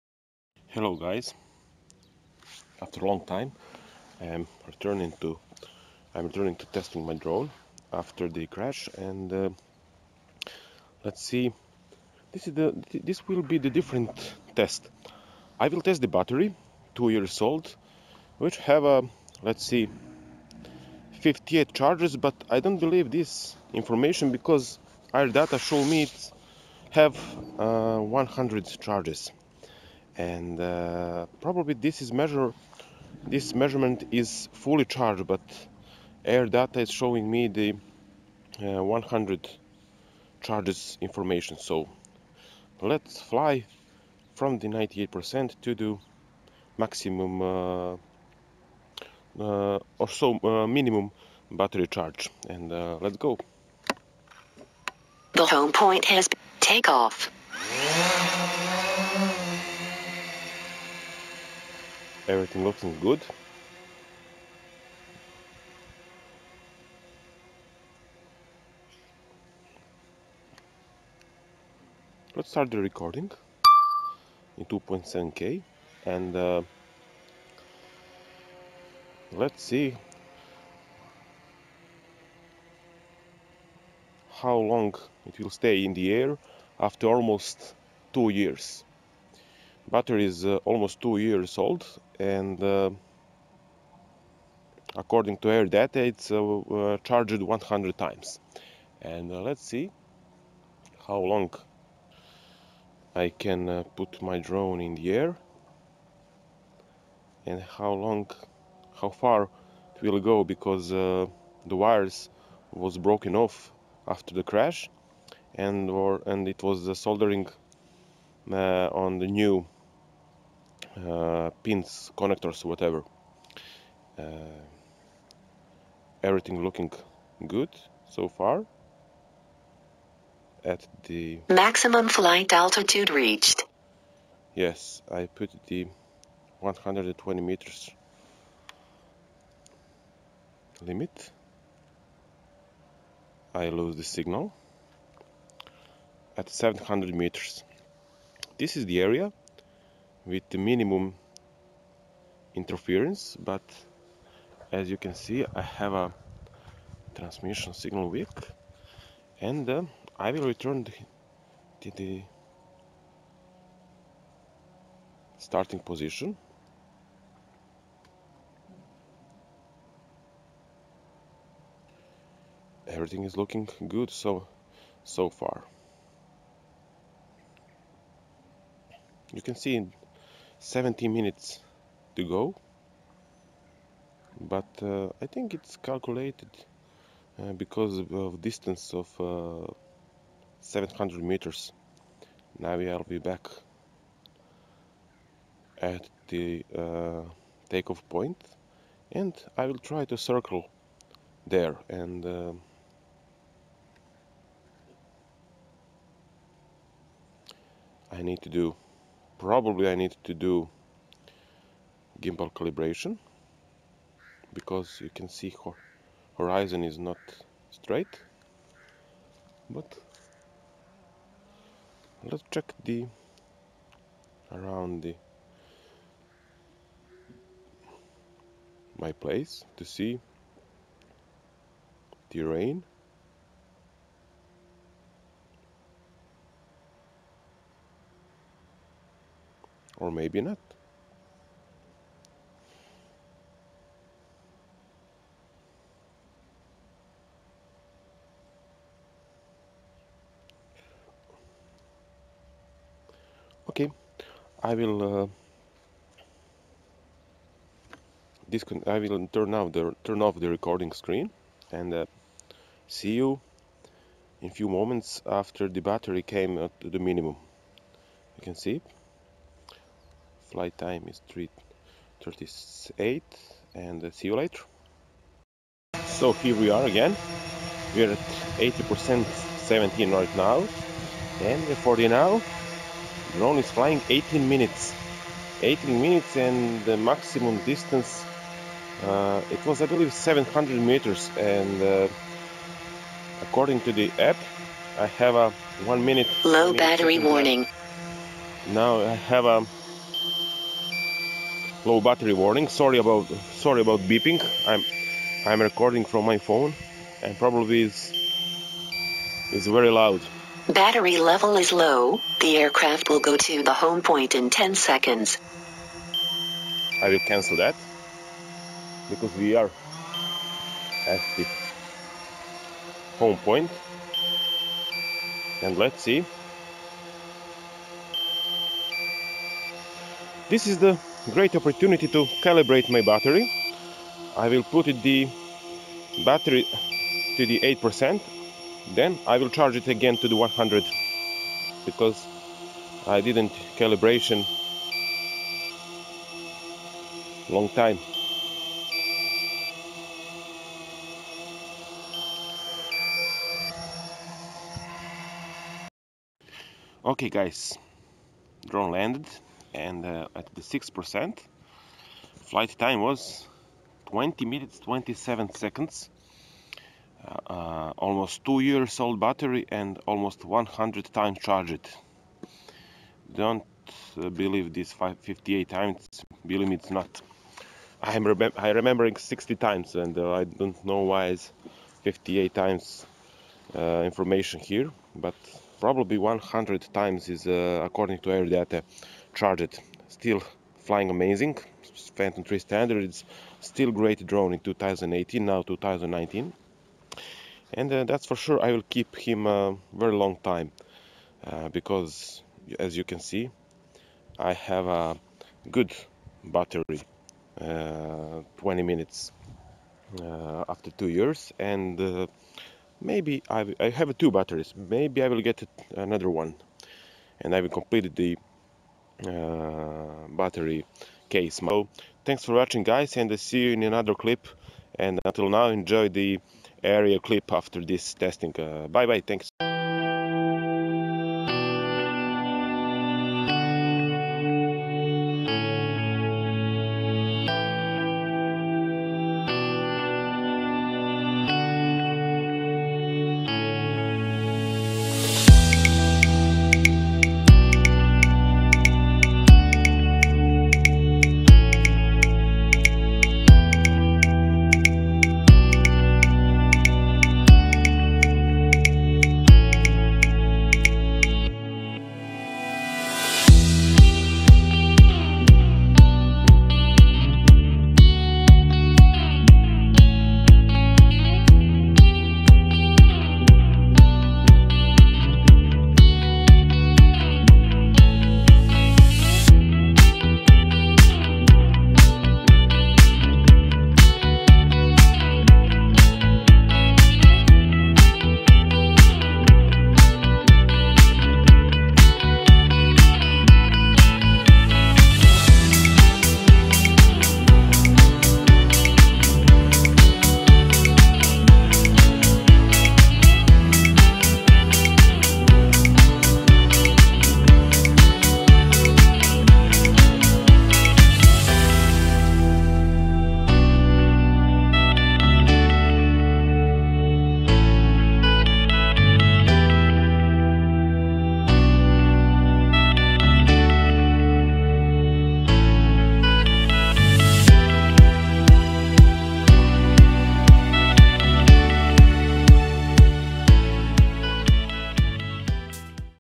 DJI Phantom 3S Battery Test after 2 Years
In this video i tested battery 2 years old and about 10 times charged. Still doing well.